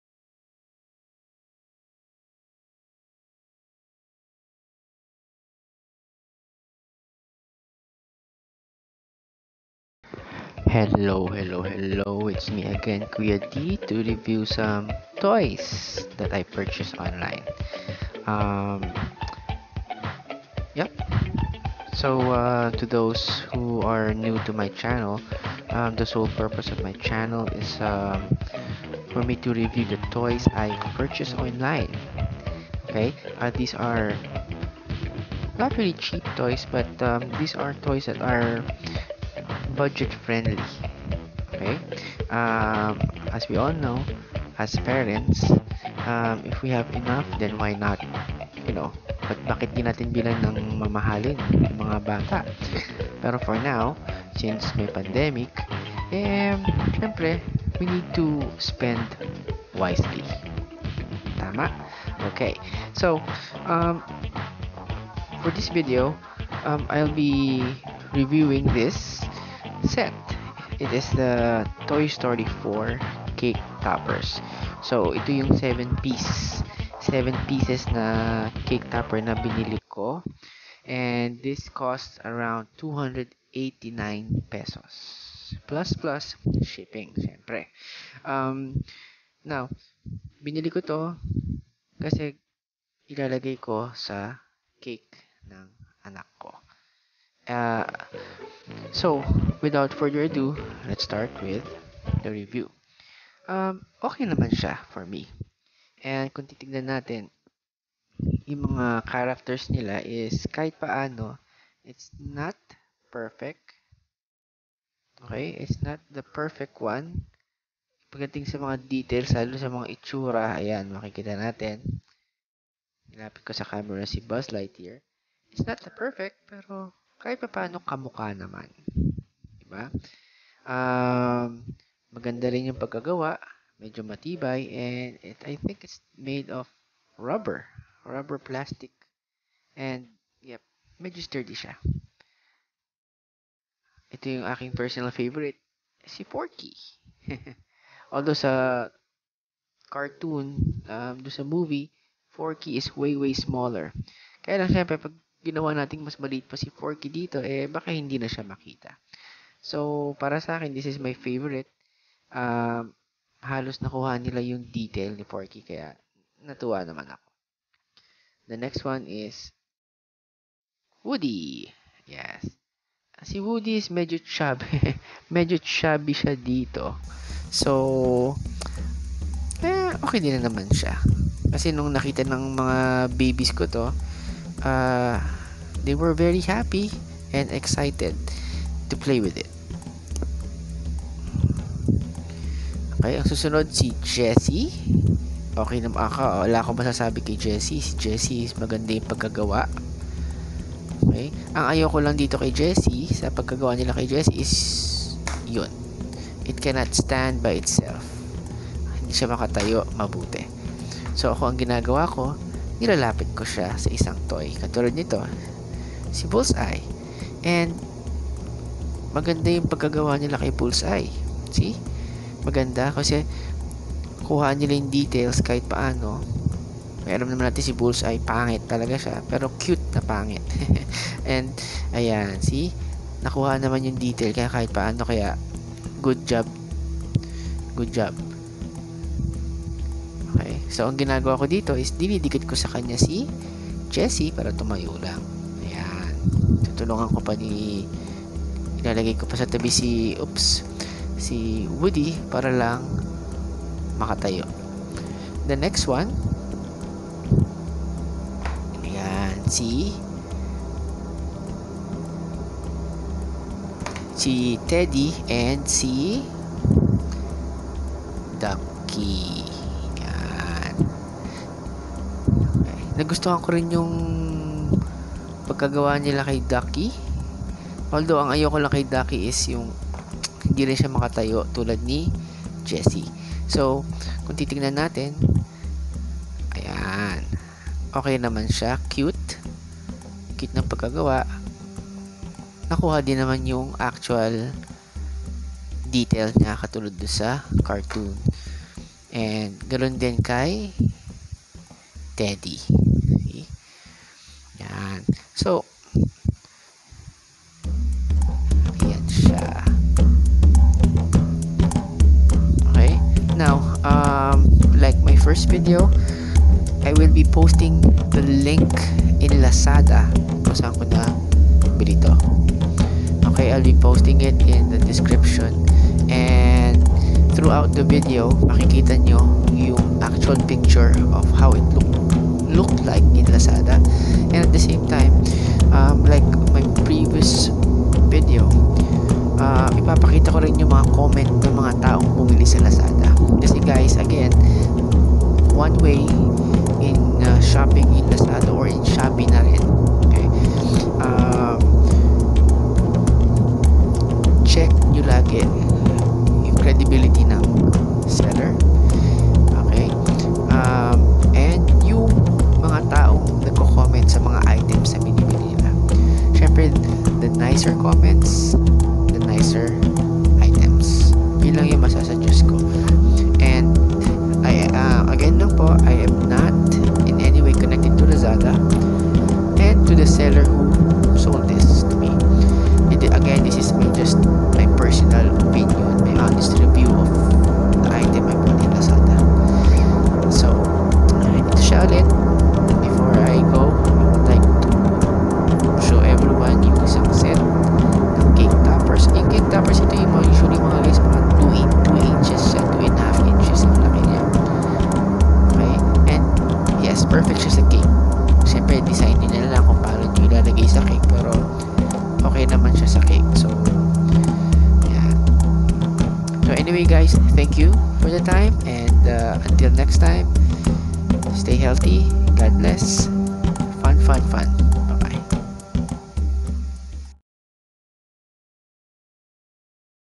Toy Story Cake Topper Review
This is my personal review and honest opinion about this Toy Story 4 cake topper.

God bless